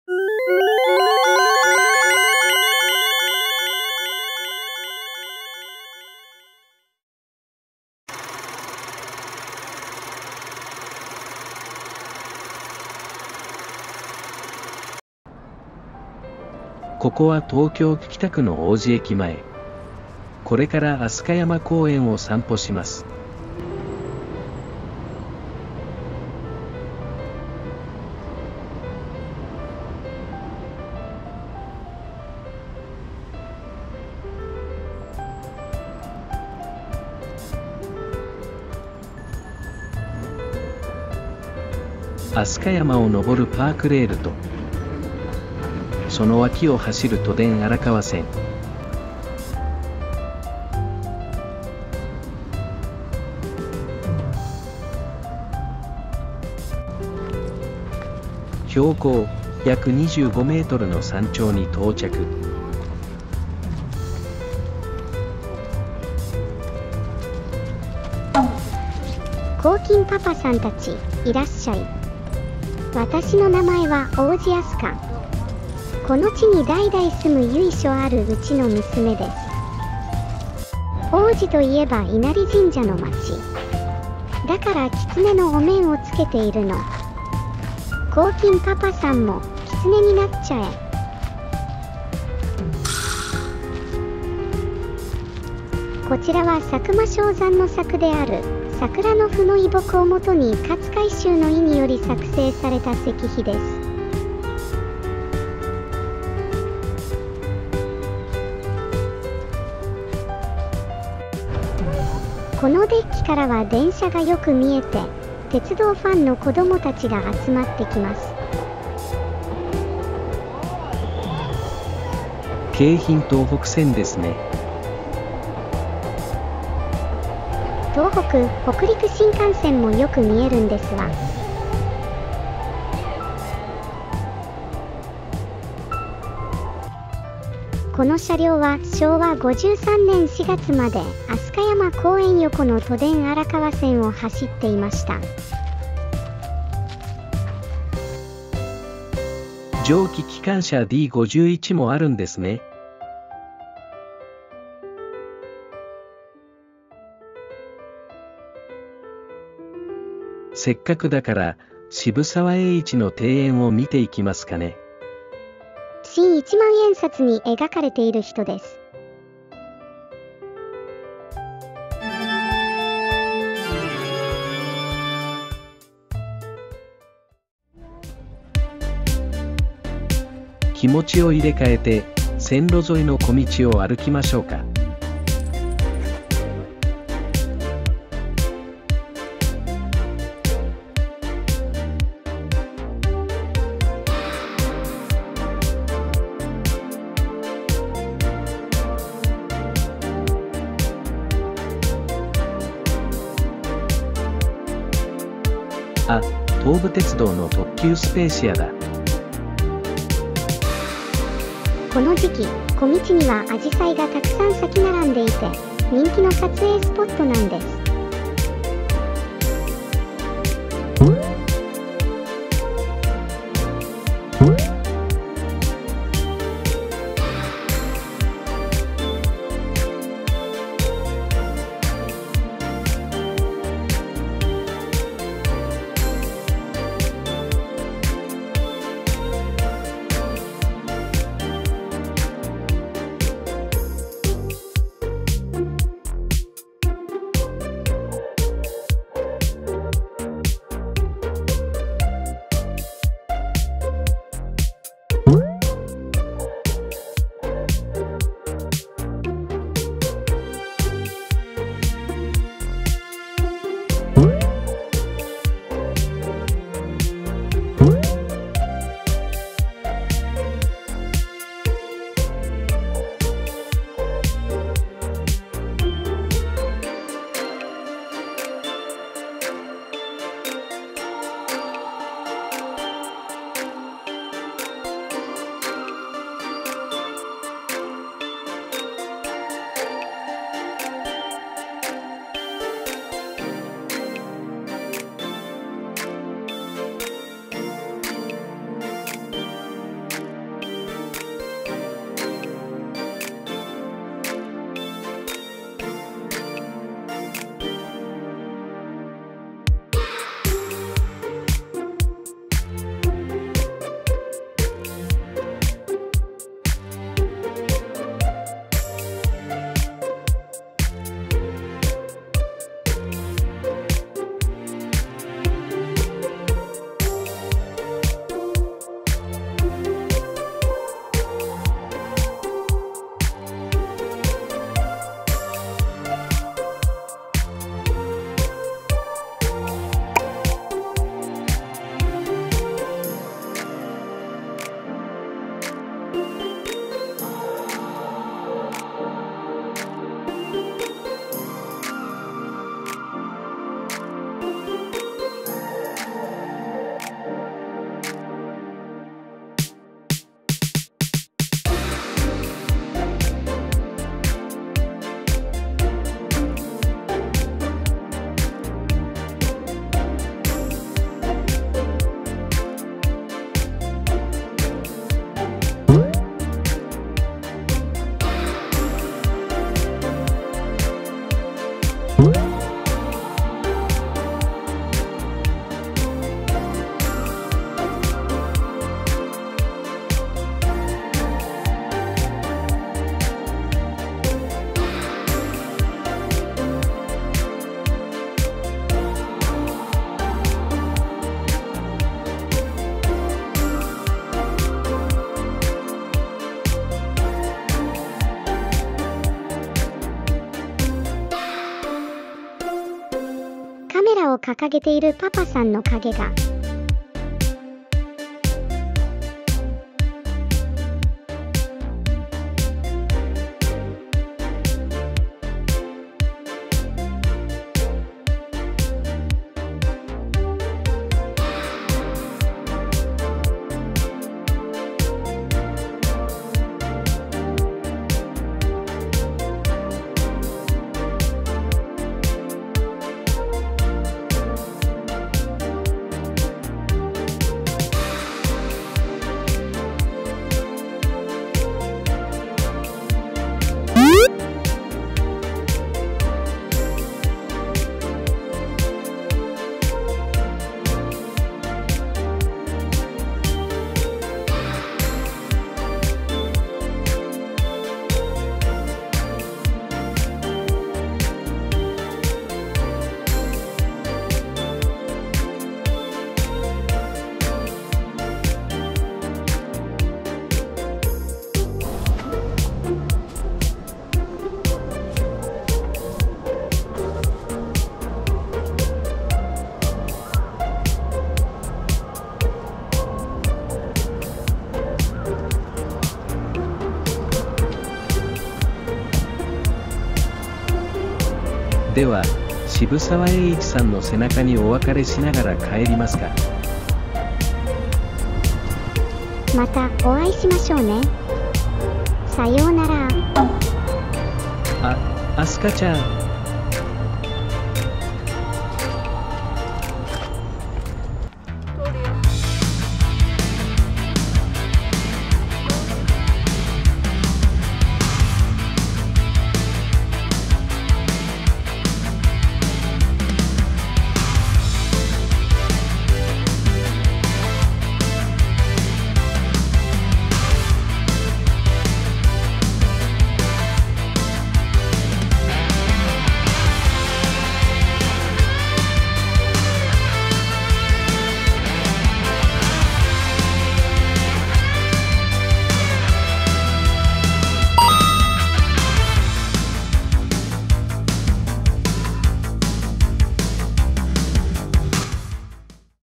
新一万円札発行記念！紫陽花の名所 飛鳥山公園を探訪／ワンワンワンワン物語２３
紫陽花の名所であり、新一万円札に描かれる渋沢栄一ゆかりの地である東京北区の飛鳥山公園を散歩してきました。王子に住む謎の少女アスカが案内してくれます。

《撮影機材》
GoPro

《フリー素材》
●音読さん

●picrew
こうきんパパ

●AIいらすと屋
渋沢栄一